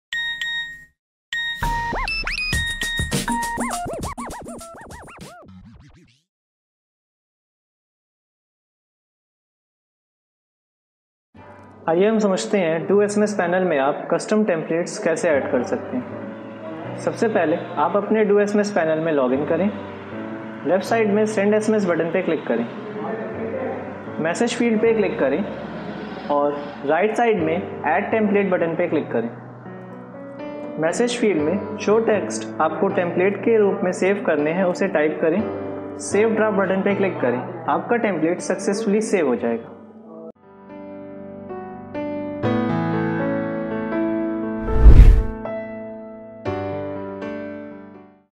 How to Add Template in DO SMS | VK SOFT
In this video, we will guide you how you can add or create templates for sending SMS in your DO SMS Account.